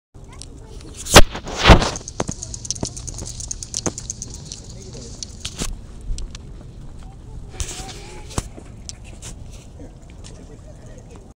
Lake Erie last April-2014
I'd has been to lake Erie in over ten years when I visited there I snapped these videos I've had on my Dell Venue pro Android HD Tablet I don't use the tablet that much only occasionally & I had ZTE smartphone with both devices that's how I captured the videos I uploaded to YouTube video last year